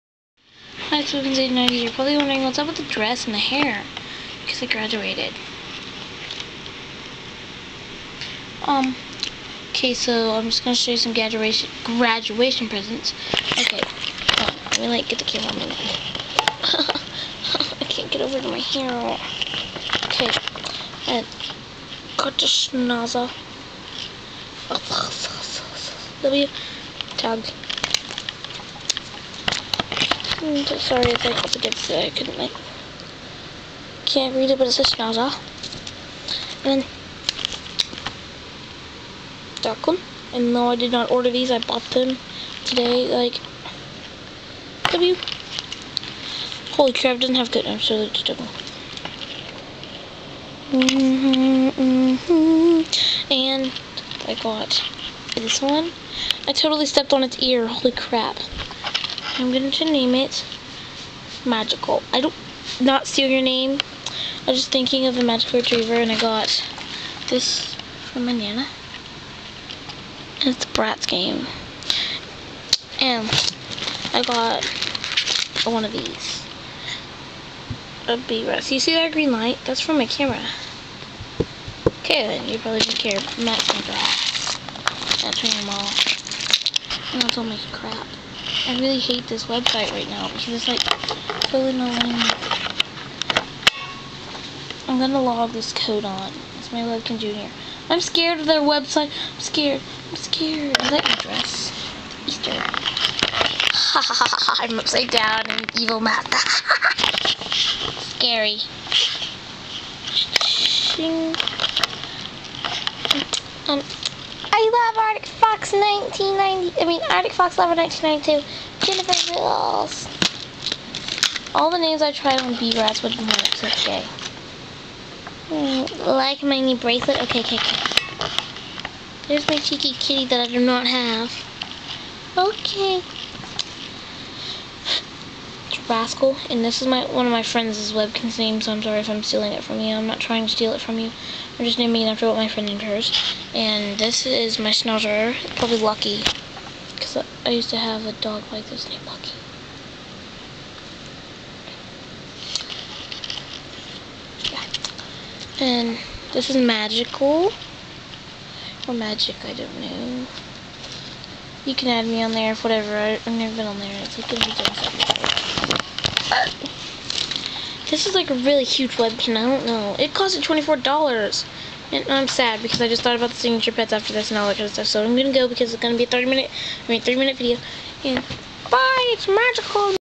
Graduation gifts
gifts I got for elementary school graduation